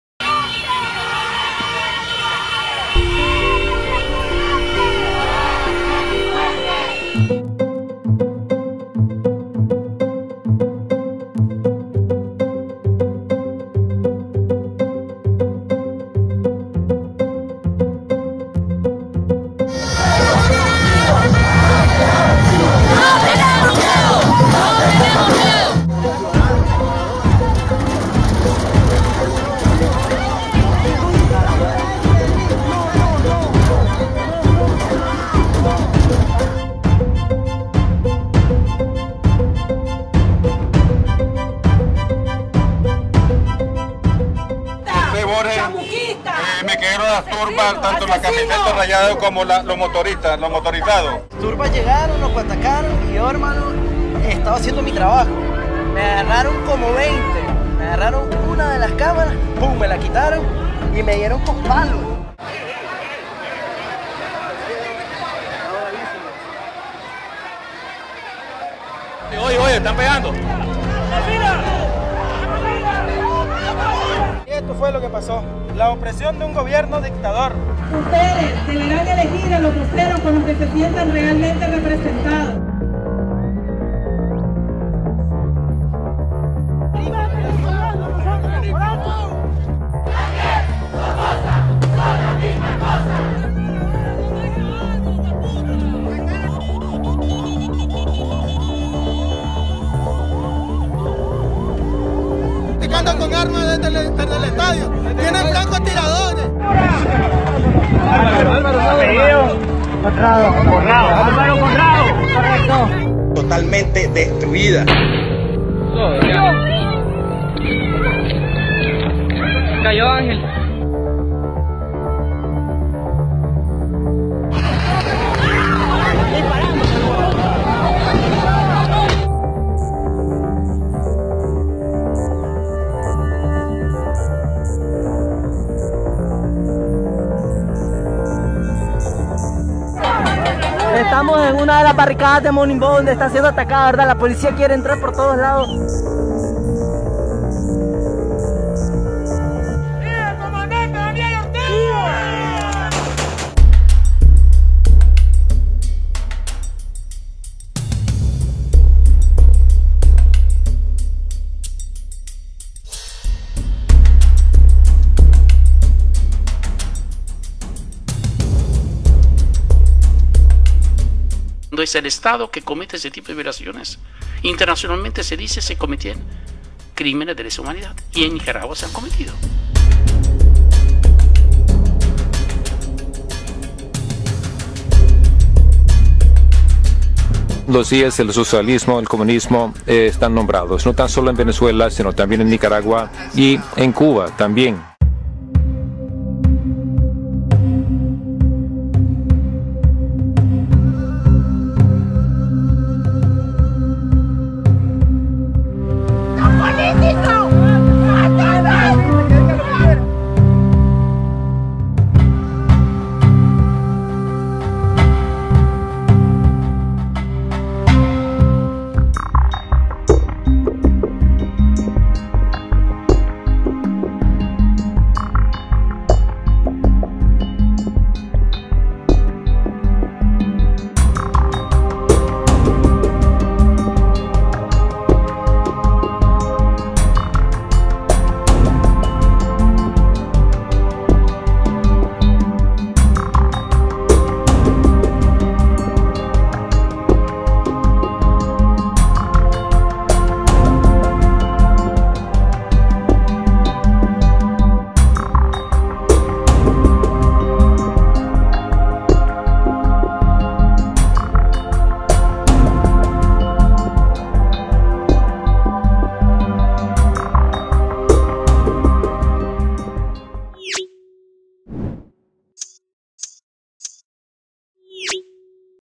Cinco claves para entender la rebelión cívica de Nicaragua
Te explicamos en cinco puntos la rebelión cívica que estalló en Nicaragua en abril 2018; y la crisis política y social que aún continúa en el país.